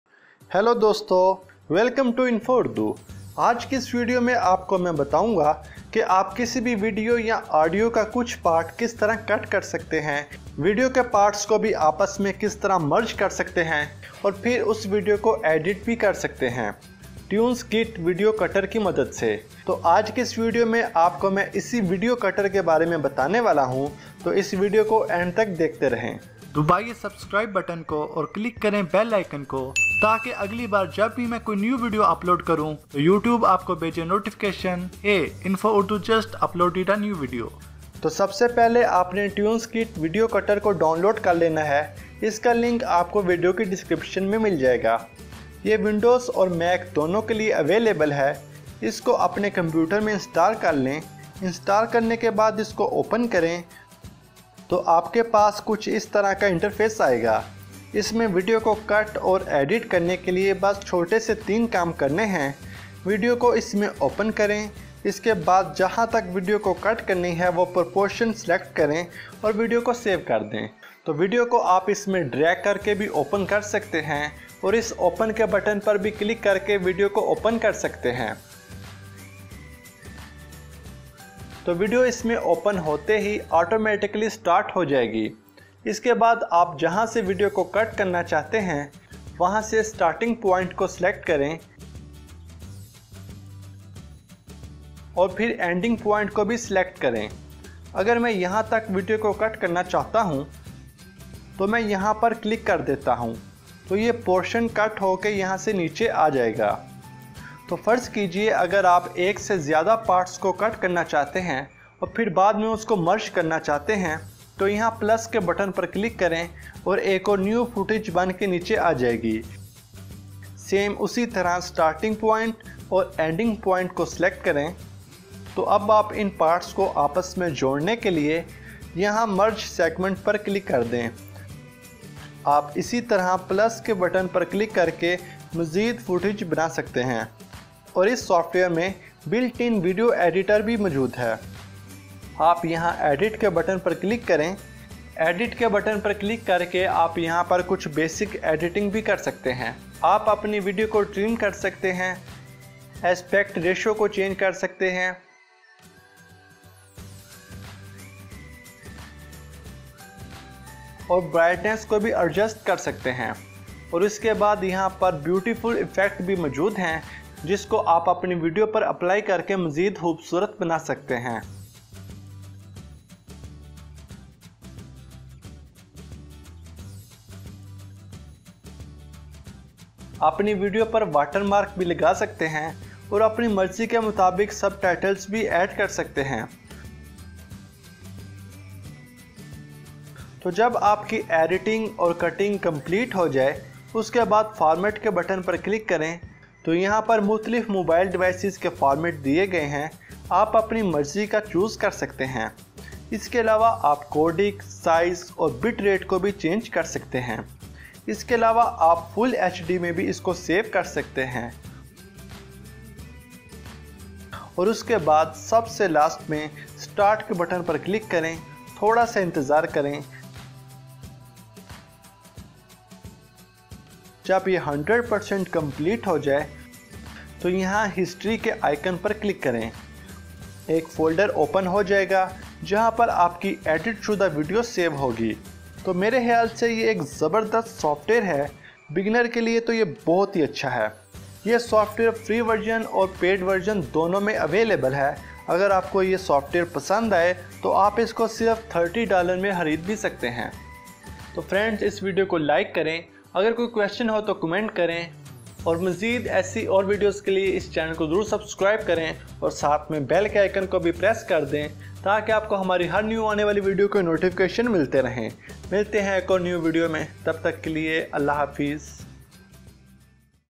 How To Trim, Cut and Edit Any Video On PC
How to cut and trim any video, audio without quality loss on PC. TunesKit Video Cutter is the best video cutter for both PC and MAC which is a lossless video splitting tool to cut, merge and edit videos for Instagram stories and whatsapp status.